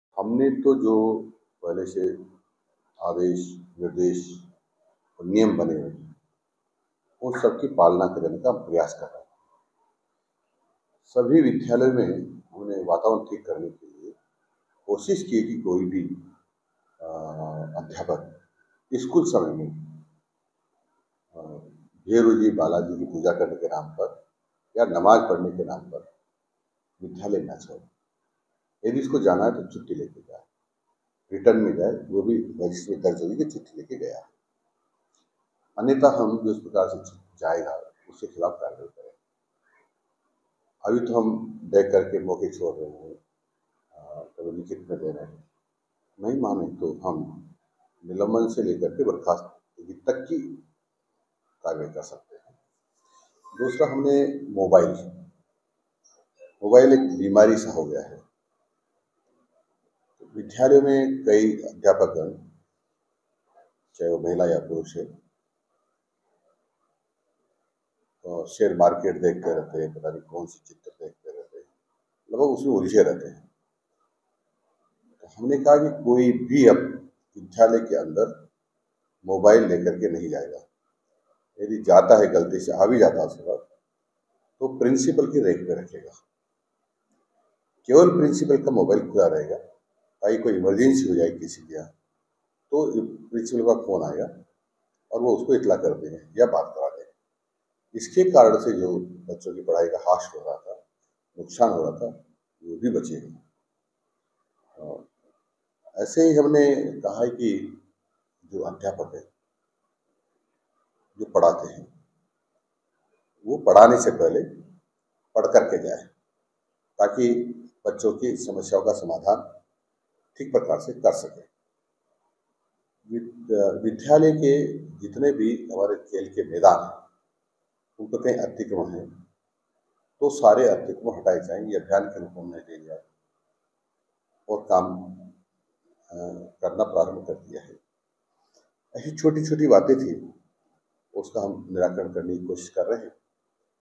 शिक्षा मंत्री Madan Dilwar का बड़ा बयान, सरकारी शिक्षक हो जाए सावधान | Rajasthan Government
About this Video:

शिक्षा मंत्री Madan Dilwar का बड़ा बयान, सरकारी शिक्षक हो जाए सावधान | Rajasthan Government | 08 May 2024

राजस्थान: "कोई भी शिक्षक स्कूल समय के दौरान बिना पूर्व सूचना के प्रार्थना या नमाज पढ़ने के लिए स्कूल परिसर से बाहर नहीं जा सकता है। बिना पूर्व सूचना दिए स्कूल छोड़ने वाले किसी भी शिक्षक को अनुशासनात्मक कार्रवाई का सामना करना पड़ेगा। इसके अतिरिक्त, शिक्षकों को स्कूल में ड्यूटी समय के दौरान मोबाइल फोन का उपयोग करने से प्रतिबंधित किया गया है।" '' ये कहना है शिक्षा मंत्री मदन दिलावर का